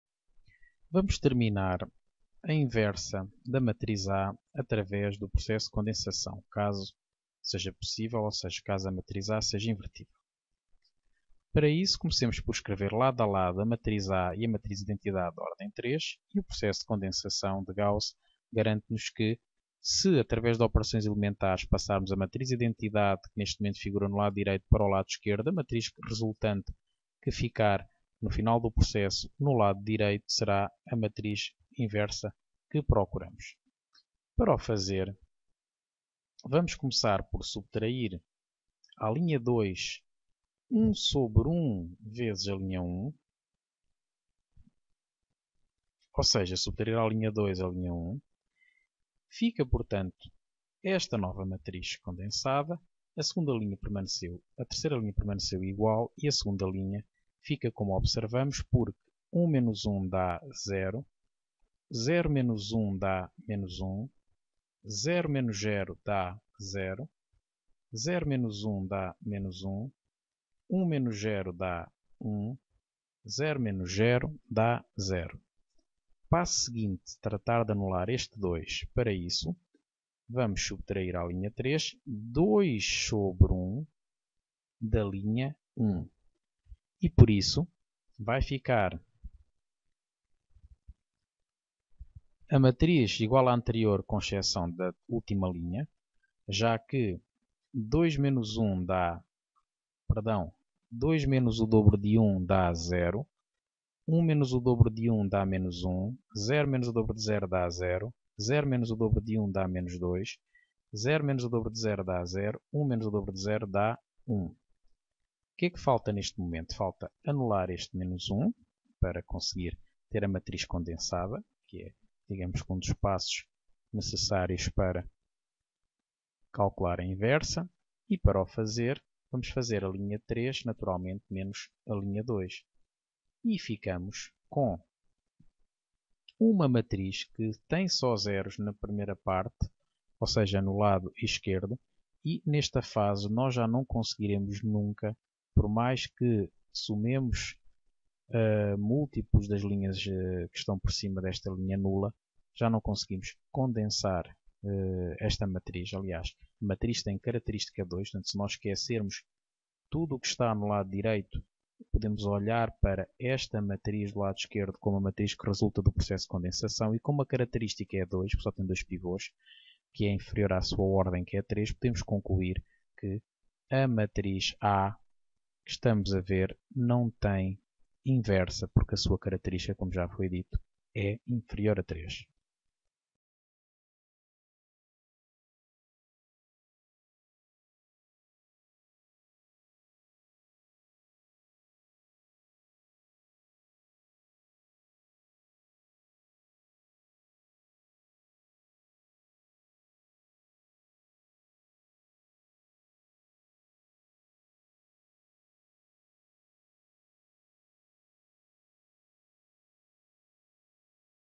Álgebra linear - determinação de uma matriz inversa usando condensação de Gauss 2
Determinamos a inversa de uma matriz 3x3 pelo Método de Eliminação de Gauss.